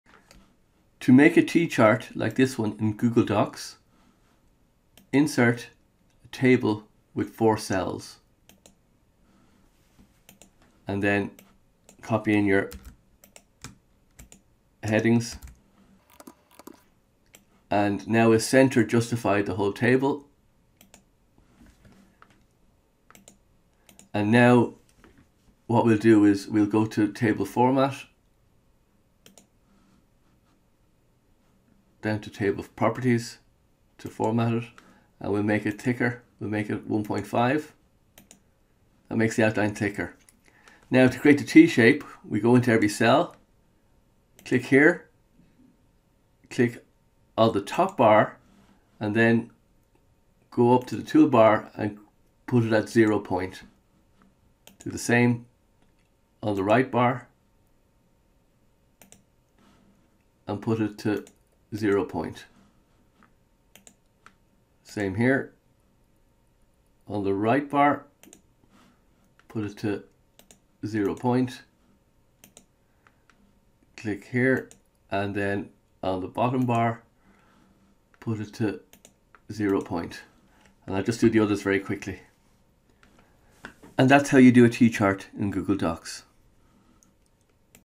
How to Make a T Chart Template in Google Docs
This video shows you how to create a T-Chart Template in Google Docs. You use a table to create a grid with 4 cells and then style the outline of each cell.